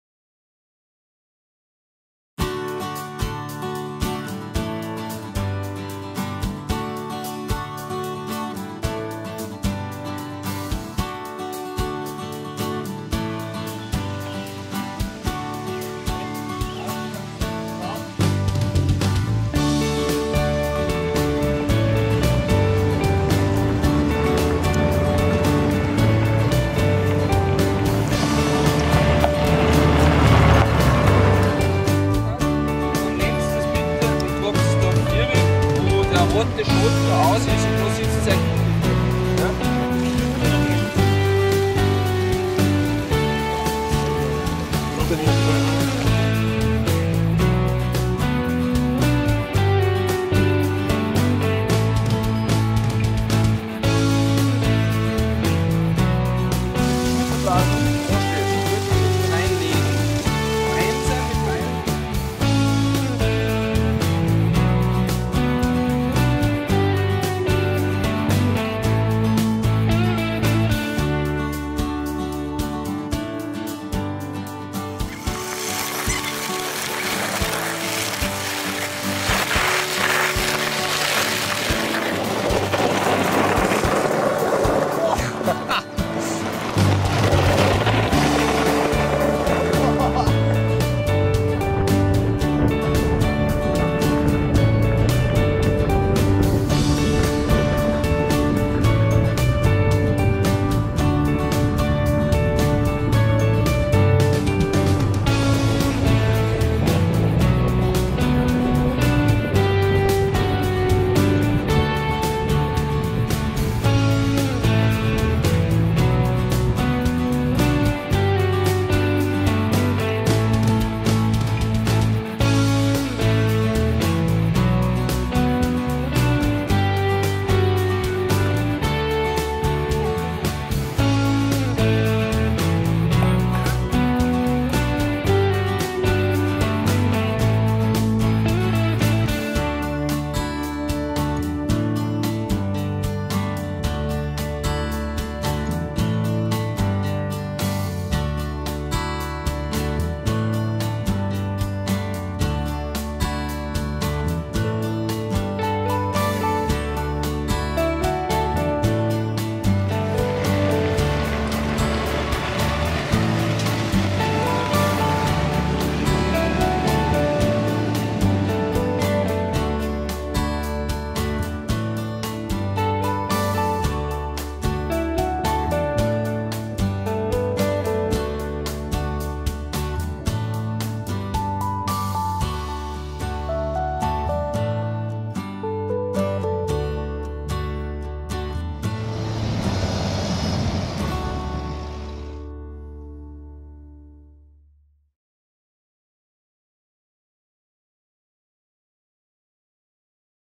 MountainCart Flachau
Mountaincart Touren mit dem Taxi auf die Alm und mit dem Mountaincart ins Tal. Mountaincart Flachau bietet grösstes Rodelerlebnis im Sommer.